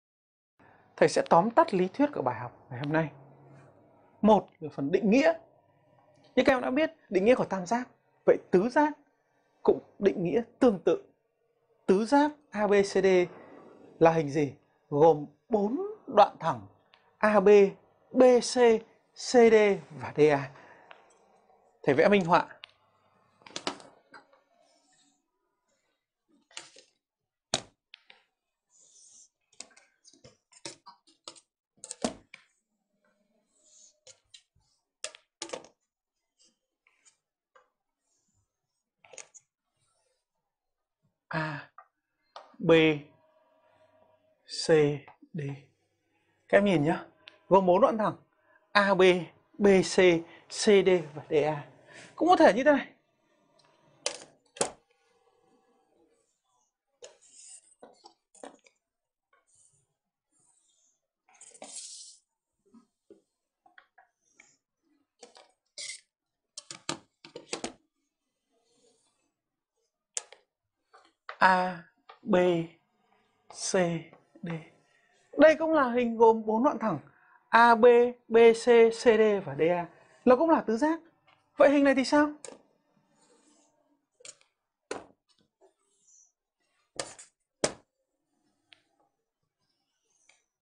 Tứ giác - Toán 8
TỨ GIÁC

1. ĐỊNH NGHĨA
Tứ giác 𝐴𝐵𝐶𝐷
Là hình gồm bốn đoạn thẳng: 𝐴𝐵,𝐵𝐶,𝐶𝐷,𝐷𝐴
Trong đó bất kì hai đoạn thẳng nào cũng không cùng nằm trên một đường thẳng

Học online Khóa học Hình học lớp 8